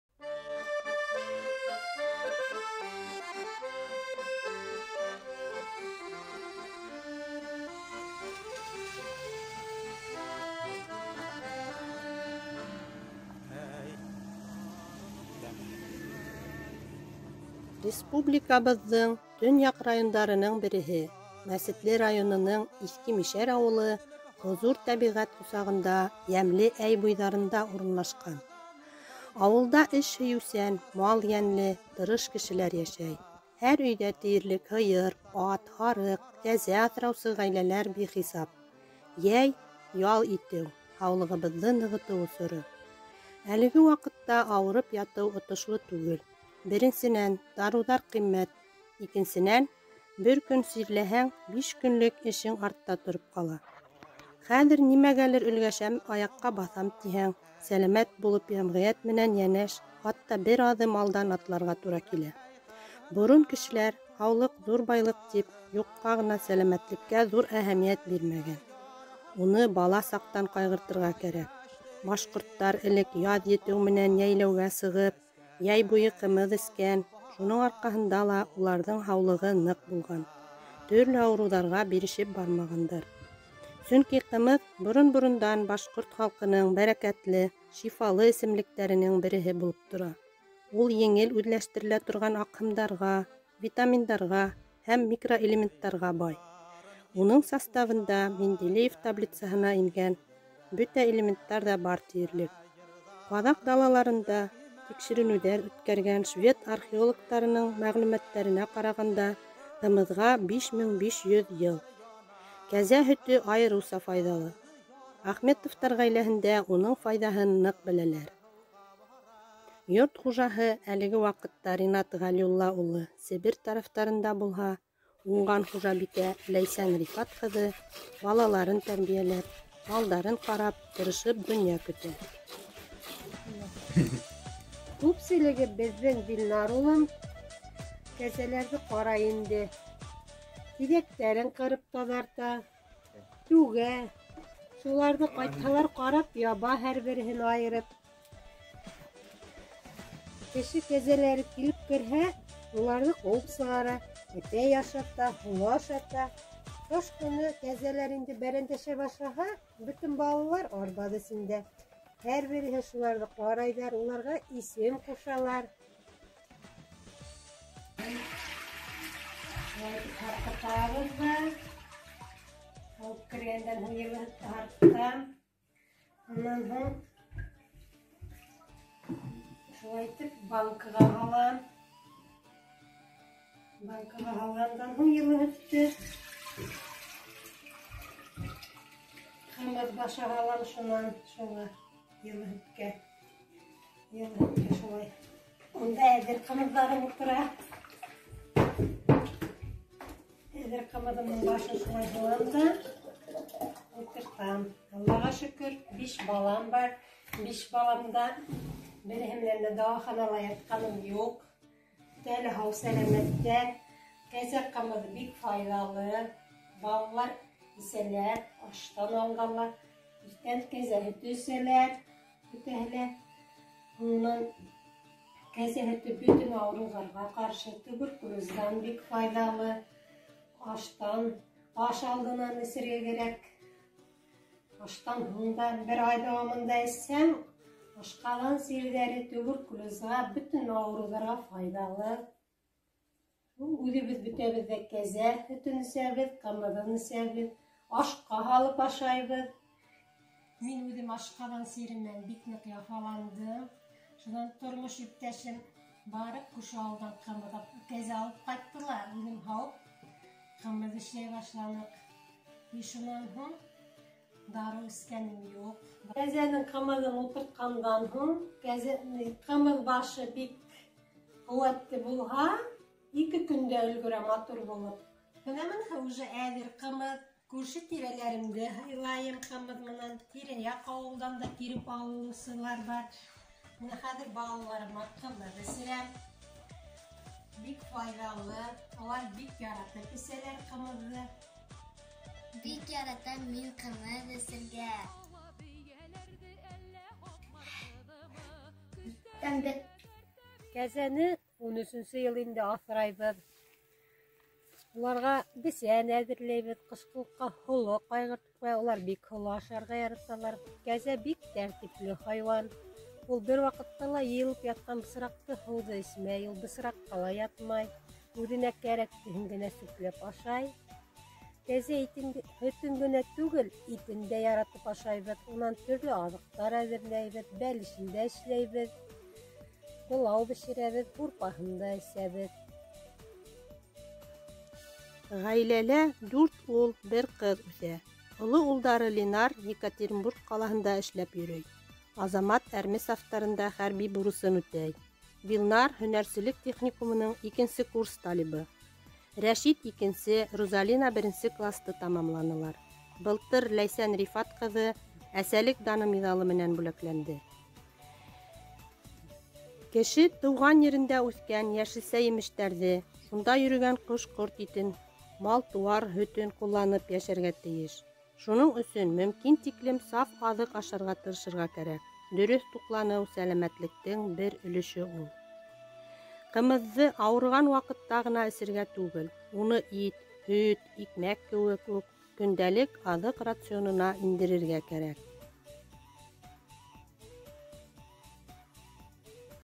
КУМЫСФЕСТ: Ахметова Лейсан Рифатовна, дер.Старомещерово Мечетлинского района
"1000 дарыу ҙа кәрәк түгел, булhaң, кәзә ҡымыҙы!"

Участник: Ахметова Лейсан Рифатовна, Дер.Старомещерово

Руководитель: Педагог дополнительного образования высшей категории, отличник образования РБ  Гарипова Фангиза Таифовна, Новояушевская ООШ, Мечетлинский район.